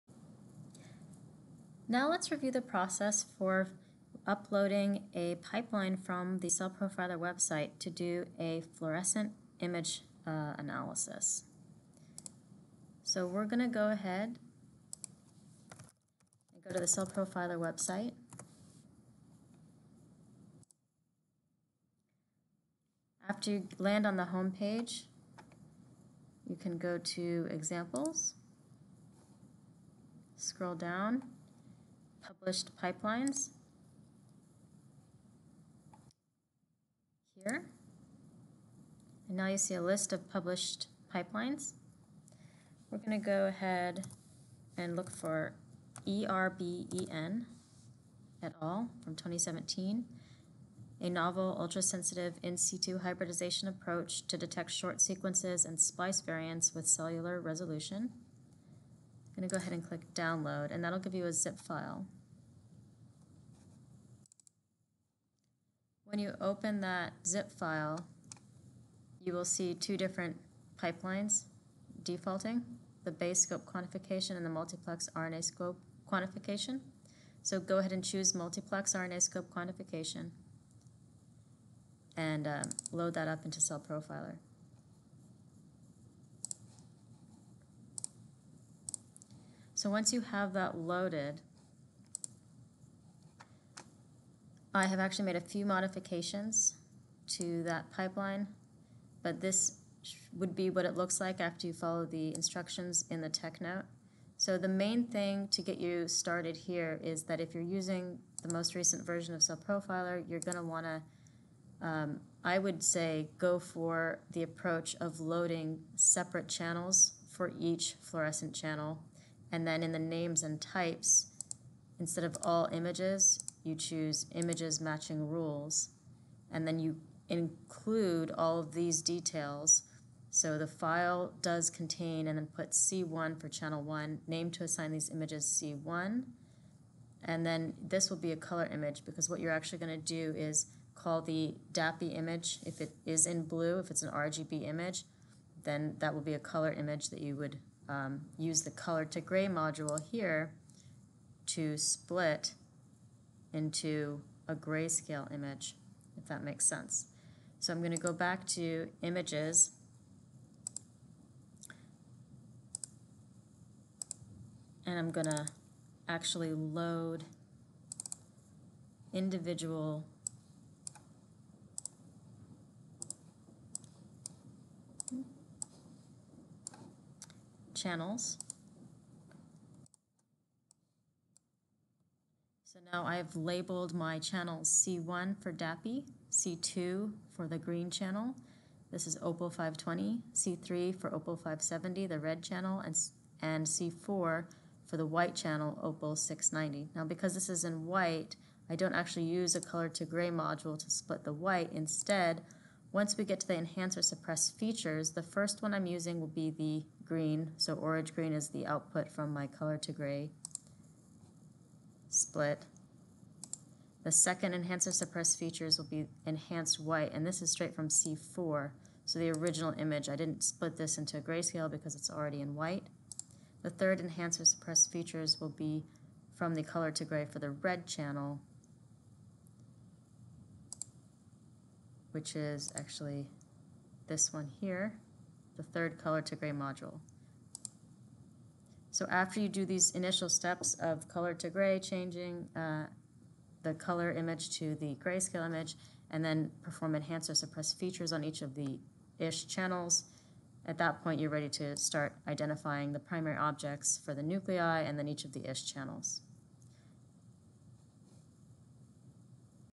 Fluorescent Assay CellProfiler Demo
From the August 24, 2019 ACD Webinar: Using CellProfiler to Analyze Your RNAscope® Images